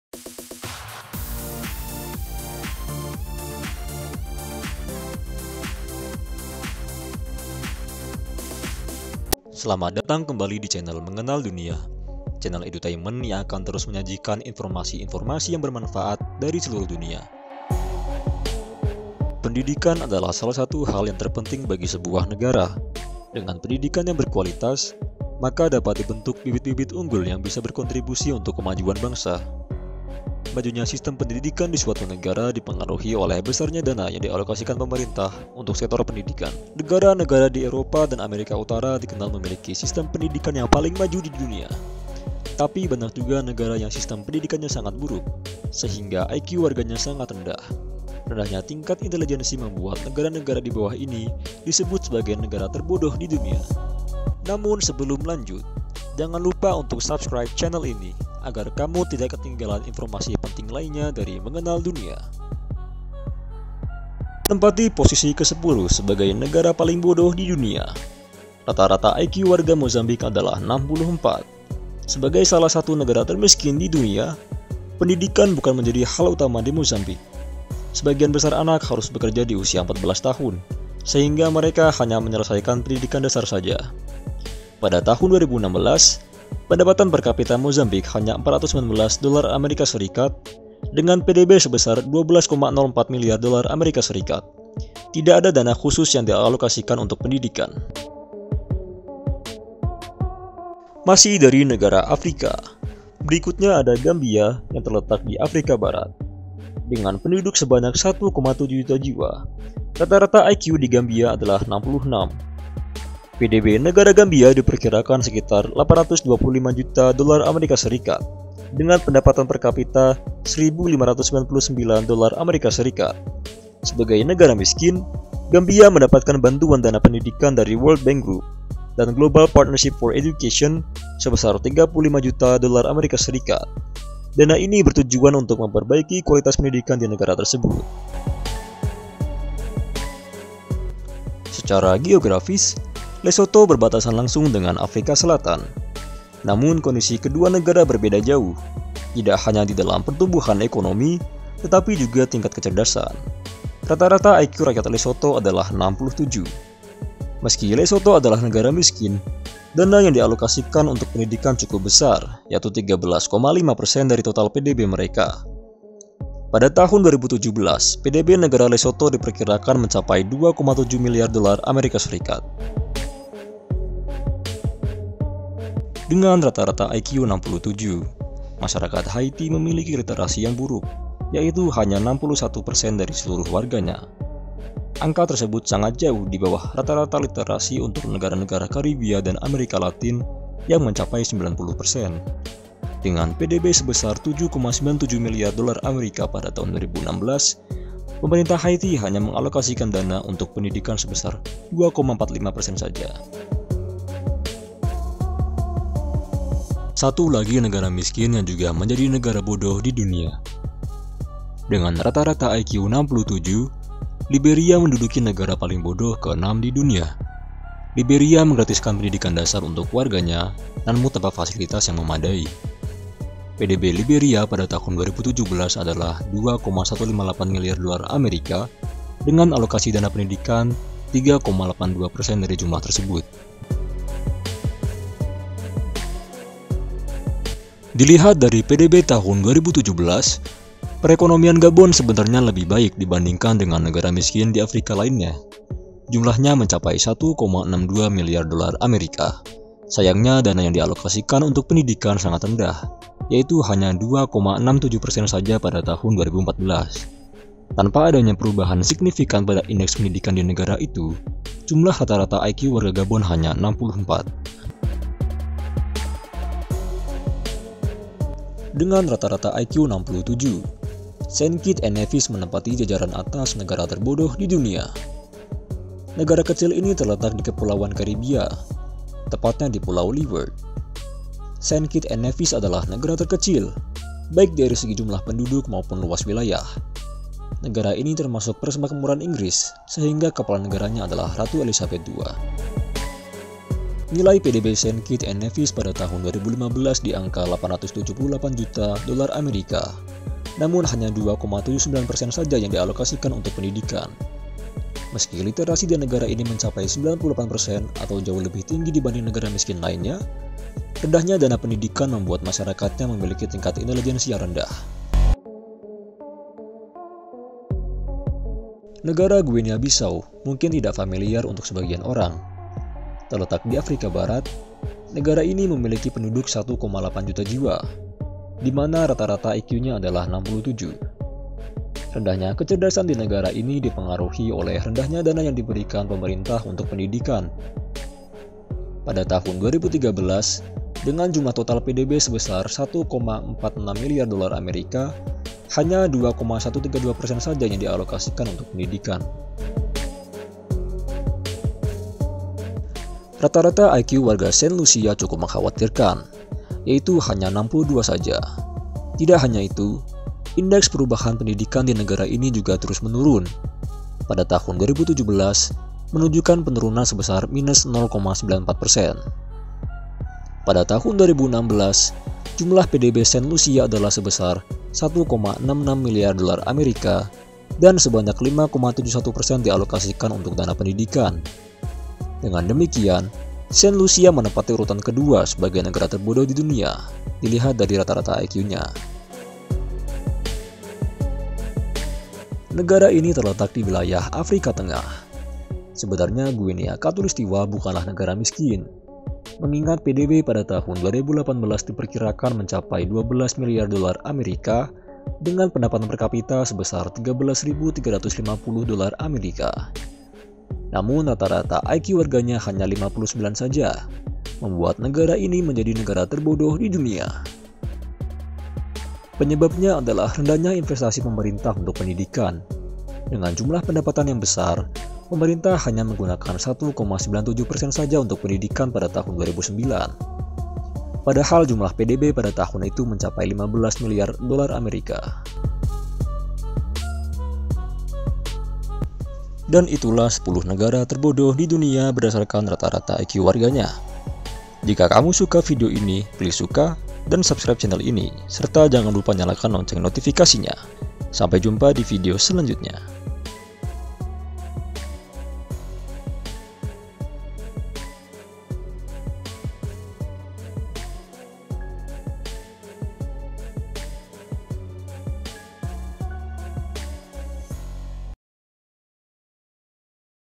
MENGENAL DUNIA - 10 NEGARA TERBODOH DI DUNIA
Fakta-fakta mengenai prestasi ataupun penemuan-penemuan banyak kita temui di negara-negara maju, namun tidak banyak yang mencoba mencari tahu bagaiamana dengan negara-negara yang memiliki keterbelakangan di bidang pendidikan. Video ini menjelaskan 10 negara terbodoh di dunia!

Eits, jangan salah paham dulu ya. Terminologi terbodoh ini didasarkan pada rata-rata IQ warga negaranya. Fakta ini di pelopori oleh Richard Lynn (Inggris) dan Tatu Vanhane (Finlandia) yang melakukan penelitian pada 80 negara pada tahun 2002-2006.

Ranking tidak hanya didasarkan pada rata-rata IQ warganya saja, akan tetapi juga terdapat faktor lain seperti anggaran pendanaan terhadap pendidikan dan termasuk kualitas serta sistem pendidikannya.

Terus pantau dan ikuti channel Mengenal Dunia ya, agar kamu tidak ketinggalan informasi bermanfaat dan menarik lainya